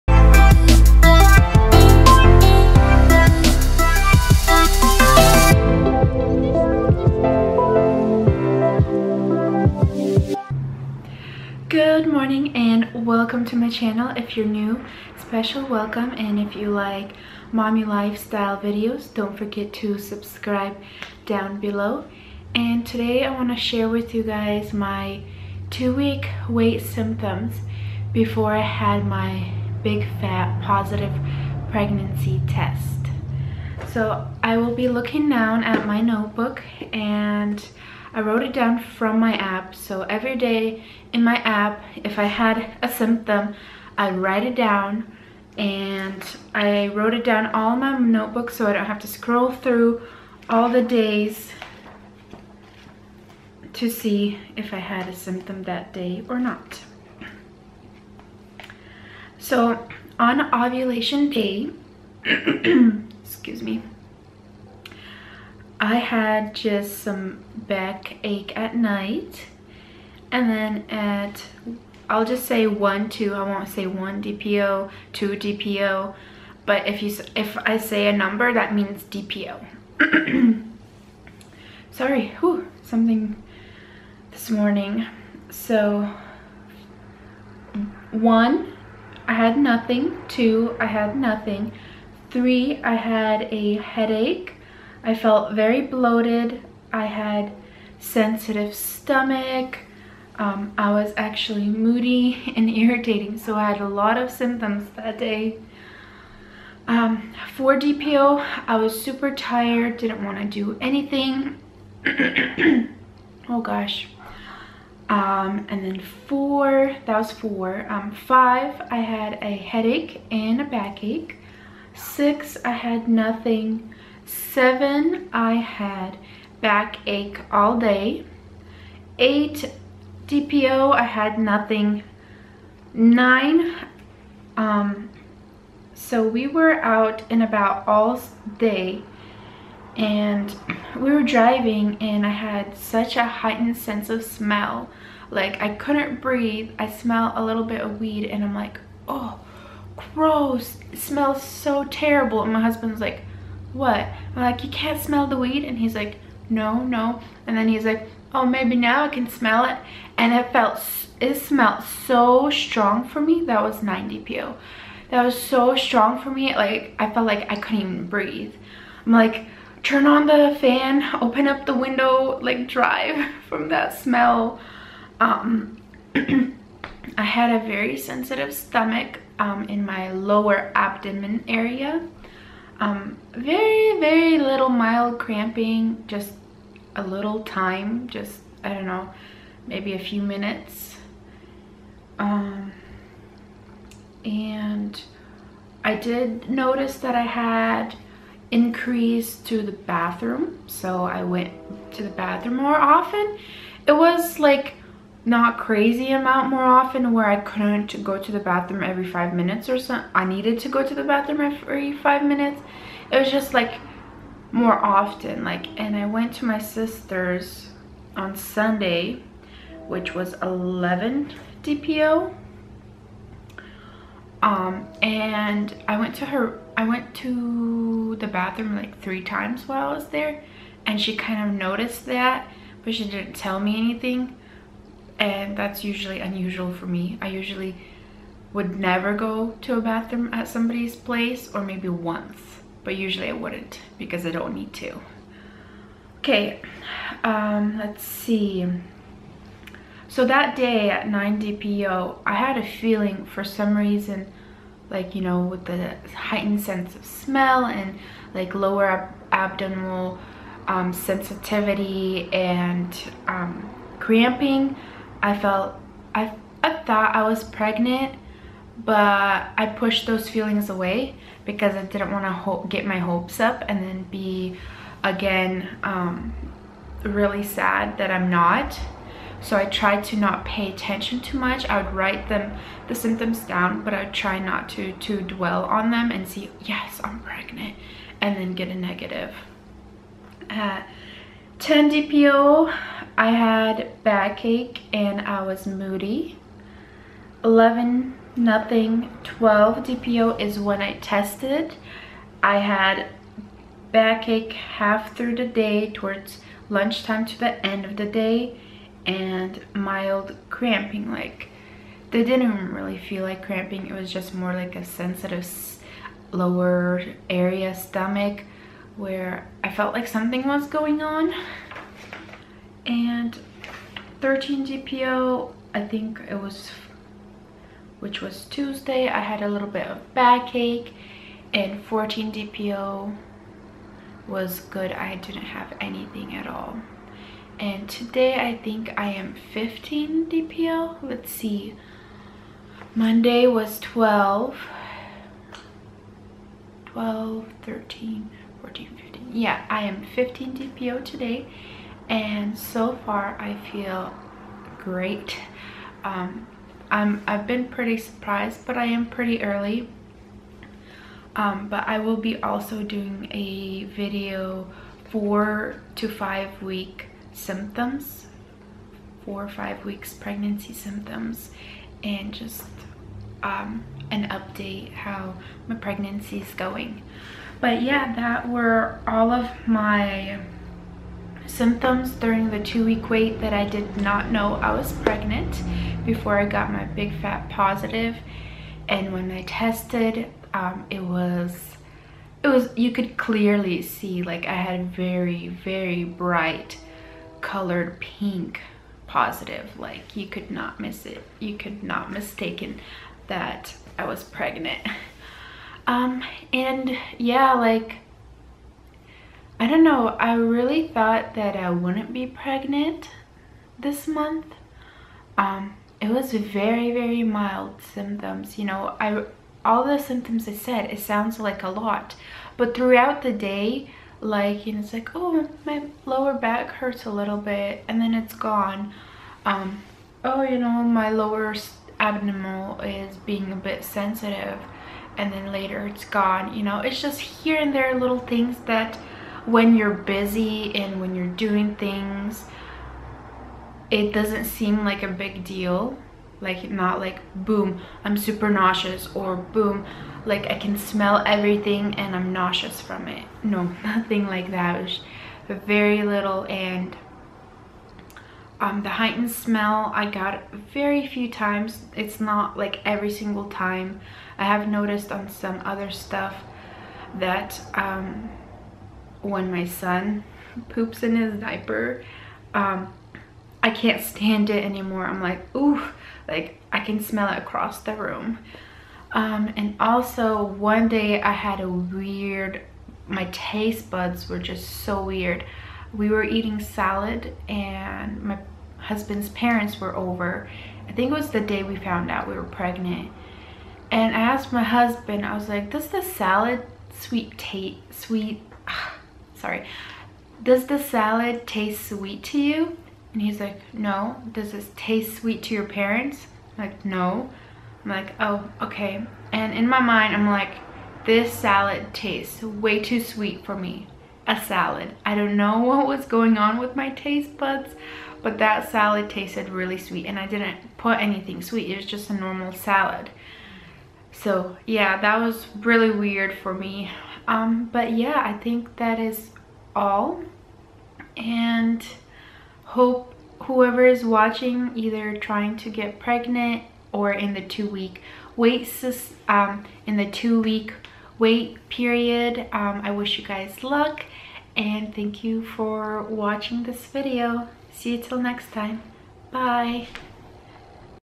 2 WEEK WAIT ~ EARLIEST PREGNANCY SYMPTOMS BFP
Hey everyone today's video is my early pregnancy symptoms in the 2 week wait before getting a big positive pregnancy test.